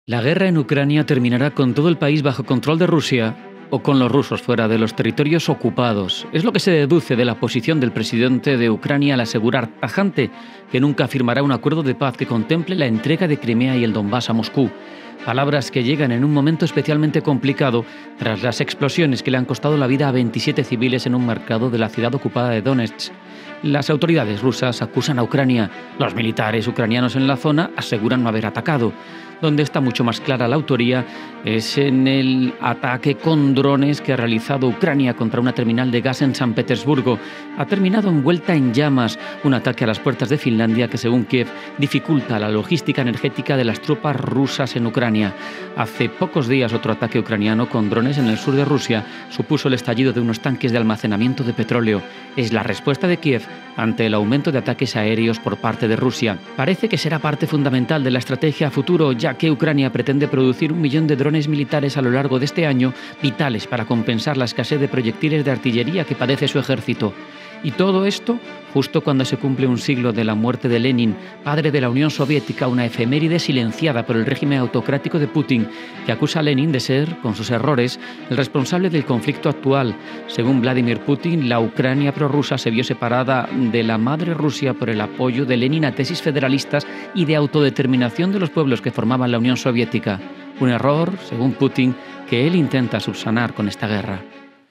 Guerra de Drones | Ucrania alcanza objetivos clave en Rusia
La guerra en Ucrania está en un punto crítico, ya que el presidente ucraniano se niega a firmar un acuerdo de paz que implique ceder Crimea y el Donbás a Rusia.

Recientes explosiones en un mercado en Donetsk generan incertidumbre sobre la responsabilidad del ataque. Además, Ucrania ha llevado a cabo ataques con drones, incluyendo uno en San Petersburgo y otro en el sur de Rusia, como respuesta a los ataques aéreos rusos.

Ucrania planea producir un millón de drones para compensar la escasez de proyectiles de artillería. En medio de todo esto, Putin culpa a Lenin por la separación de Ucrania de Rusia, considerando su apoyo a la autodeterminación como el origen del conflicto actual.